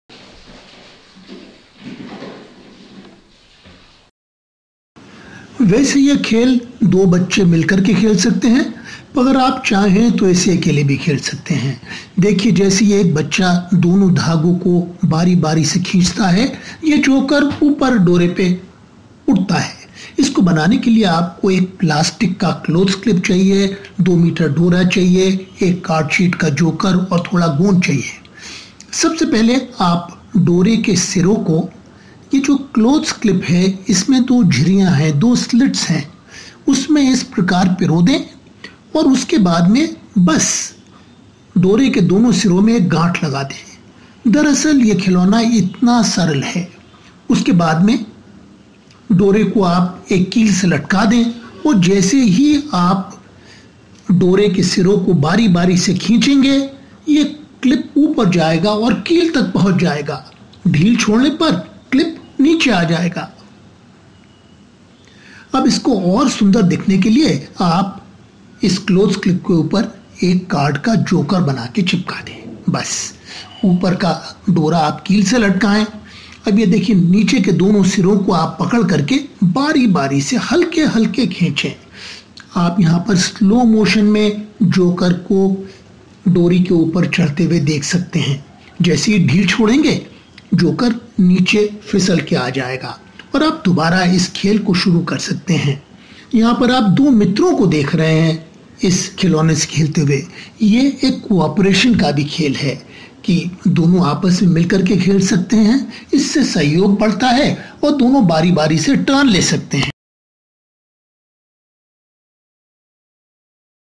Climbing Clothes Clip | Hindi
This is a game for two, though you can play it alone too. Here you can see a plastic Clothes Clip climb up. As the child pull the two end threads alternately, the Clip climbs up the thread. To make this you will require a plastic clothes clip, card sheet joker, glue and 2-meters of thread. Weave the thread in the slits of the Clothes Clip as shown. Then tie a knot at the two ends. Hang the thread by a nail in the wall. Now hold the two ends of the thread and pull them alternately. Slowly, you will see the clip climb up the wall. Now stick a picture of a Joker to the Clothes Clip. Hang the top of the thread by a nail in the wall. As you pull the two ends of the thread alternately, the Joker will climb up the thread. Here you can see the climb in slow motion. On releasing the tension in the threads the clip and Joker will fall. You can start the game again. Two friends can play it together in a game of cooperation. One holds the top of the thread and the other pulls the end threads alternately. They can then take turns.